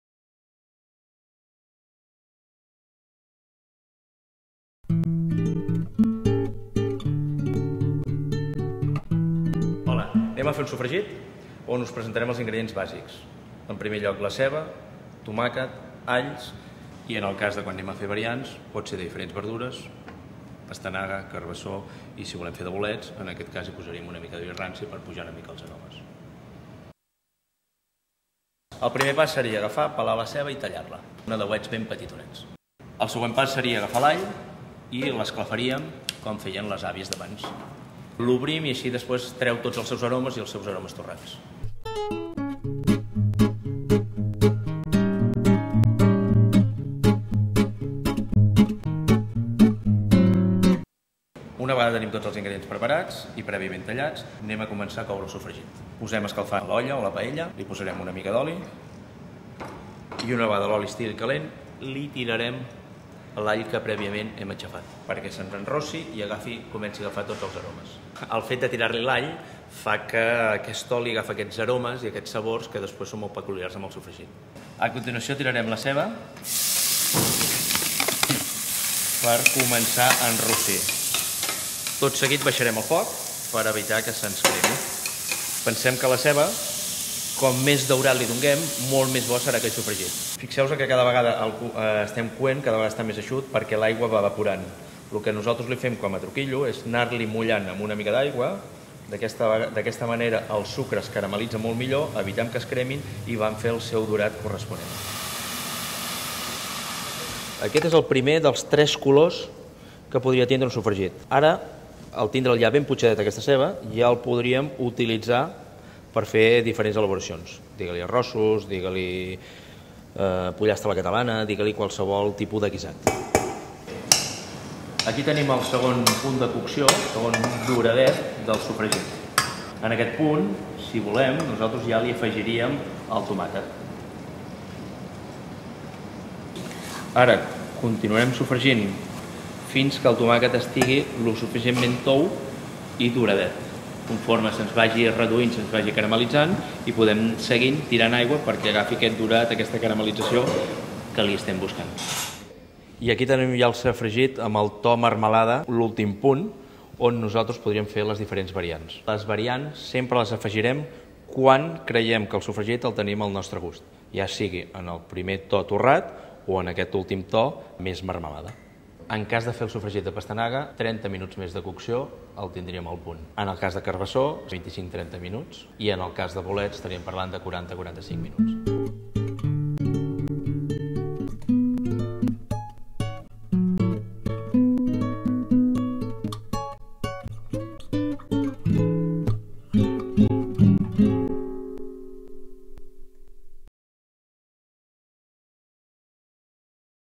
RECEPTA: EL SOFREGIT
El cuiner del CETT Oscar Teixidó uis ensenya a fer un sofregit de professional.